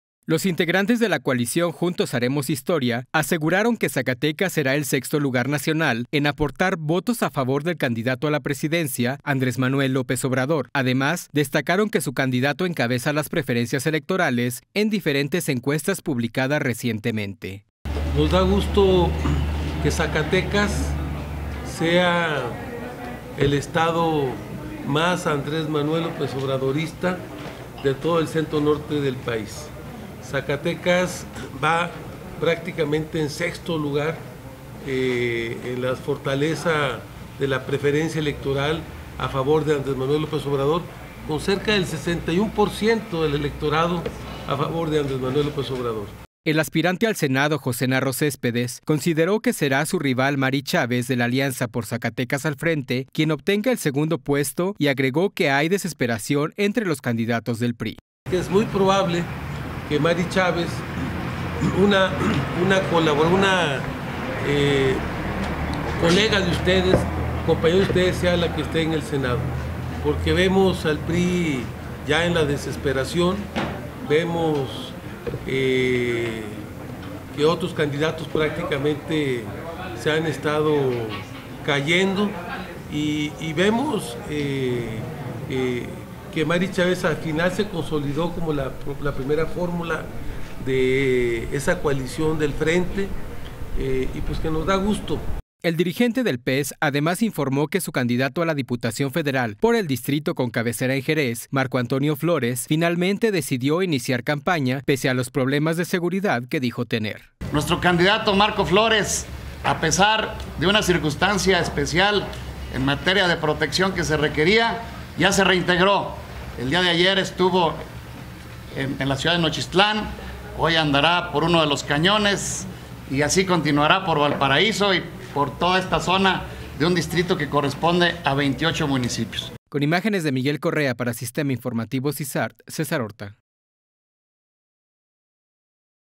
Conferencia
Los integrantes de la coalición Juntos Haremos Historia (Morenal- PT-PES), aseguraron que Zacatecas será el sexto lugar nacional en aportar votos a favor del candidato a la Presidencia, Andrés Manuel López Obrador.